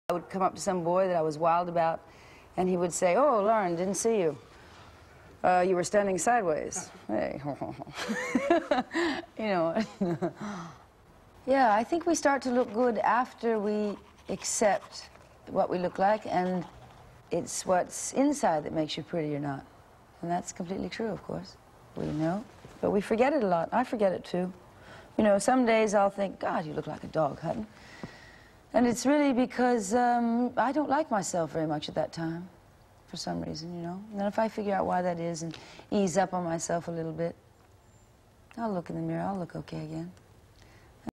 1980s USA, Lauren Hutton on Beauty
1980s USA, Lauren Hutton on Beauty from the Kinolibrary Archive Film Collections. Clip ref AB2 . For commercial projects only. To order the clip clean and high res, or to find out more, Available in SD.

1980s, Lauren Hutton on Beauty.